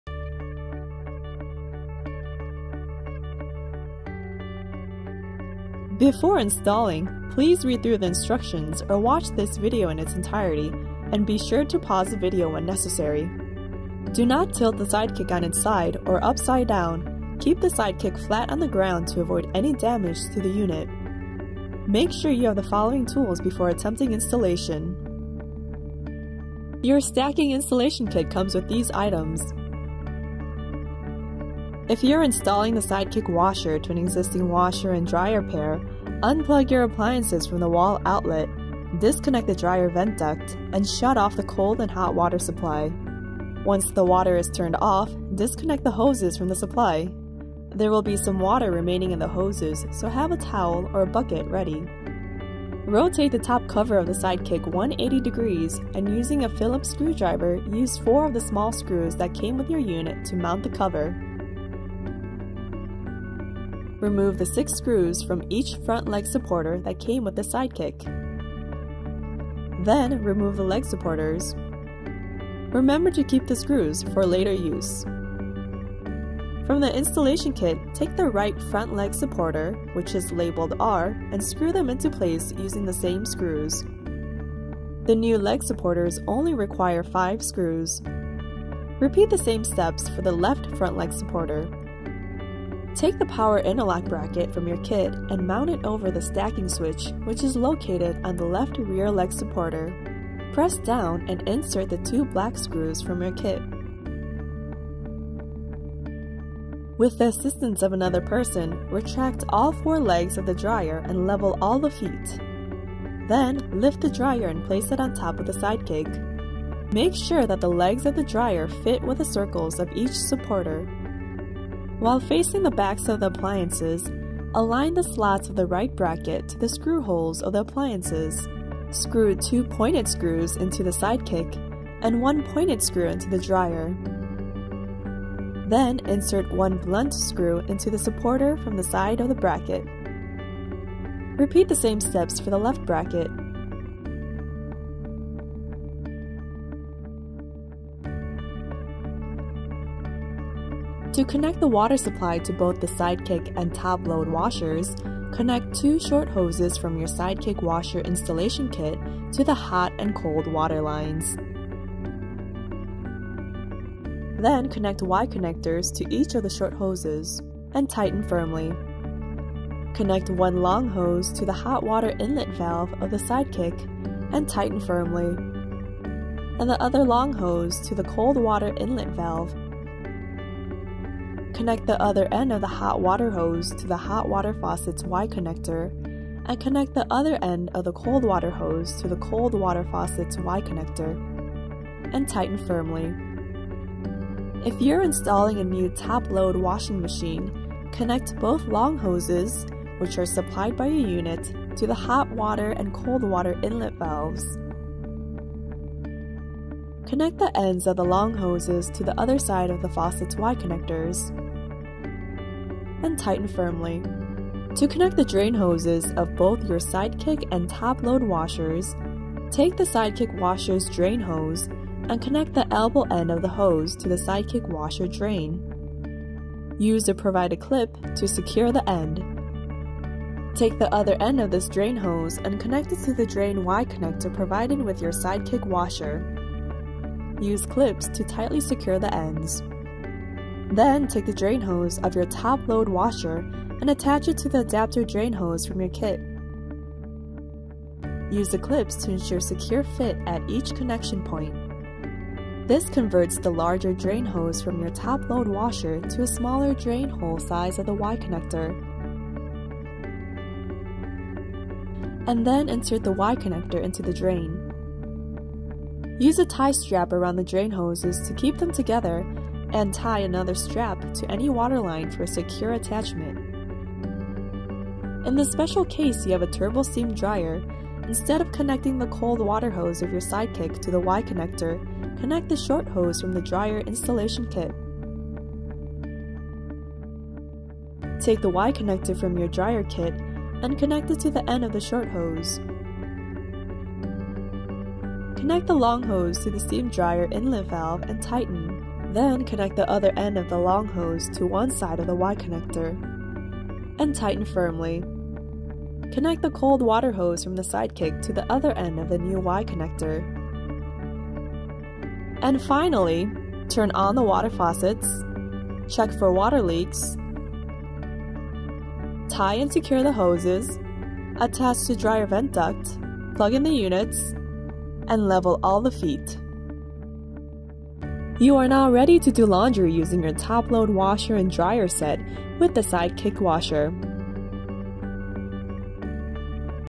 LG SideKick™ Installation Under a Dryer (Top Load Situation)
Your LG Sidekick™ washer acts as an additional mini-washer for your LG Washer – allowing you to wash two loads at once. This video will show you how to install your LG Sidekick™ under a dryer.